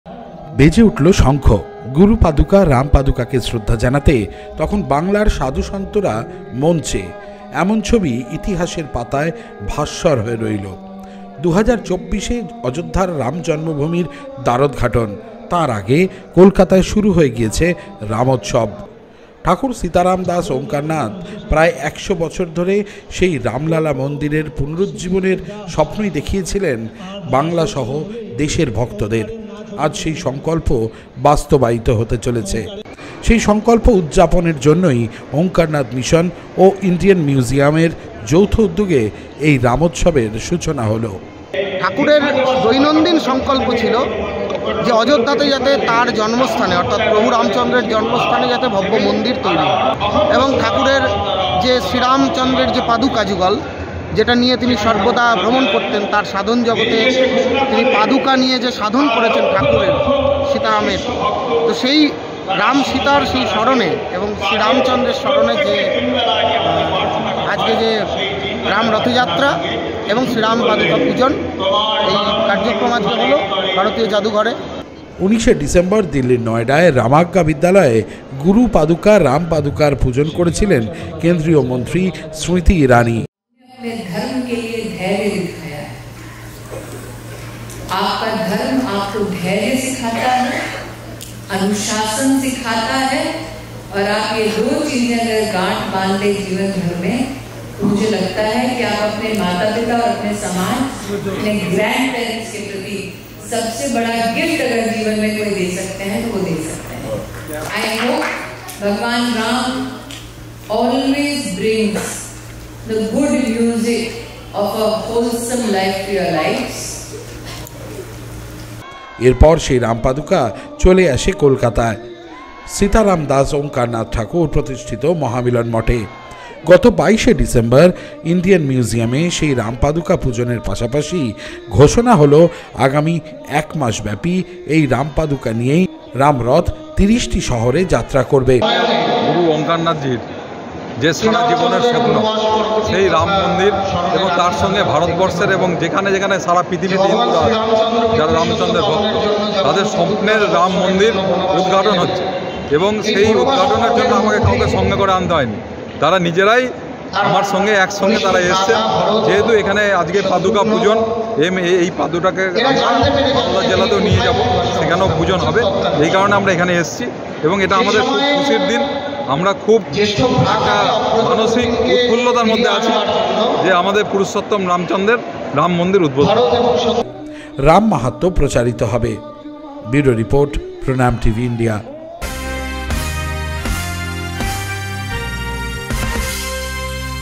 গুরু-পাদুকা রাম - পাদুকা এলো কলকাতায়! ভারতীয় জাদুঘরে শ্রদ্ধা জানালেন সনাতনীরা
শ্রী রাম উৎসব । মর্যাদা  পুরুষোত্তম, শ্রী রামের পুন্য জন্মভুমি – অযোধ্যায় প্রান প্রতিষ্ঠিত হতে চলেছে ২২ শে জানুয়ারি ২০২৪। ৫০০ বছরের সনাতনীদের নিত্য সংঘর্ষের ইতিহাস, স্বর্নাক্ষরে লেখা আছে ইতিহাসের পাতায়।
ঠাকুর সীতারাম দাস ওঙ্কারনাথ বাংলার অন্যতম মণিষী বিগত ১০০ বছর ধরে নিত্য প্রার্থনা করে গেছেন, শ্রী রাম্ম জন্মভুমি প্রতিষ্ঠার। সেই সঙ্কল্প পূর্তির উৎসব ওঙ্কারনাথ মিশনের রাম উৎসব।
ঠাকুর সীতারাম দাস ওঙ্কারনাথ । তাঁর নামেই রয়েছে সীতা ও রাম। সনাতনী ভারতের শাস্ত্রজ্ঞ, মহাজ্ঞানী এই দিব্য পুরুষ এর সাহচর্যে এসেছেন সারা দেশের অগনিত  ভক্ত , শিষ্য। এমন কি সারা দেশ জুড়ে রয়েছে তাঁর শতাধিক আশ্রম – মঠ – মিশন।
সারা দেশে ছড়িয়ে থাকা তাঁর অগ্নিত ভক্ত তাঁকে স্বয়ং রাম অবতারের রূপ বলেই মনে করে থাকেন।
সনাতনী ভারতের গুরু শিষ্য পরম্পরায় তিনি লব্ধ করেছিলেন তাঁর গুরুর পারম্পরিক বেদ – উপনিষদ সহ ভারতীয় শাস্ত্রের সম্যক জ্ঞান।
তাঁর গুরু জ্ঞানের সংগে তিনি লব্ধ করলেন,আরেক পারম্পরিক  দুর্মূল্য অর্ঘ্য্। পরম্পরা অনুযায়ী সেই
“গুরু – পাদুকা”- “রাম-পাদুকা”।সেই পাদুকা বুকে নিয়ে তিনি আসমুদ্র-হিমাচল পরিভ্রমুন করেছেন।
আজও সেই “গুরু – পাদুকা, রাম-পাদুকা” হৃদয়ে নিয়ে আছেন কিংকর বিঠঠল মহারাজ।
গত ১৯শে ডিসেম্বর  তারিখ দিল্লির নয়ডায় সেই “গুরু-পাদুকা-রাম পাদুকা” কে বিশেষ পুজো অর্চনের মধ্যে দিয়ে শ্রদ্ধা জানালেন কেন্দ্রীয় মন্ত্রী স্মৃতি ইরাণি।
২১ তারিখ দুপুর ১ টা নাগাদ  সেই অমূল্য পাআদুকা ও আসন বিমানে দমদম নিয়ে আসা হয় মহামিলন ওমকারনাথ  মঠে।
২২ তারিখ সেই  অমূল্য পাদুকা নিয়ে যাওয়া হয়েছিল   ইন্ডিয়ান মিউজিয়ামে। সেখানে ইন্ডিয়ান মিউজিয়ামের আধিকারিকদের পাশাপাশি উপস্থিত ছিলেন বাংলার বিভিন্ন ধর্মীয় সংগঠনের সাধু -সন্তরা ।
সারা দেশে রাম নাম মাহাত্ম্য প্রচার ও আগামী প্রজন্মকে রাম নামের ও সনাতন ভারতের ইতিহাসকে পুনুরুজ্জীবিত করার লক্ষে রাম- রথ এই পাদুকা নিয়ে পরিব্রজে থাকবে আগামী এক মাস । দেশের প্রায় ৩০ টি শহরে।
আজ যখন অযোধ্যায় রাম-মন্দির উদবোধনের সময় এগিয়ে আসছে, সে প্রসংগে বলা প্রয়োজন রাজনৈতিক ভাবে নয় , অযোধ্যায় রাম লালার মন্দিরের খানিক দূরেই রয়েছে সীতারাম দাস ওঙ্কারনাথ মঠের আরেকটি কেন্দ্র।
যে কেন্দ্র থেকে ঠাকুর সীতারাম ওঙ্কারনাথ ঈশ্বরের কাছে তাঁর শিষ্য দের নিয়ে রাম জন্মভুমি পুনরুদ্ধারের প্রার্থনা জানাতেন।
সেই সংগে অরাজনৈতিক ধর্মীয় আন্দোলনের পুরোধা ছিলেন তিনিই। আজও তাআই সেই মন্দিরের যিনি প্রধান সেবায়েত নিযুক্ত হয়েছেন তাঁর গুরু ছিলেন  ঠাকুরের একান্ত ভক্ত ।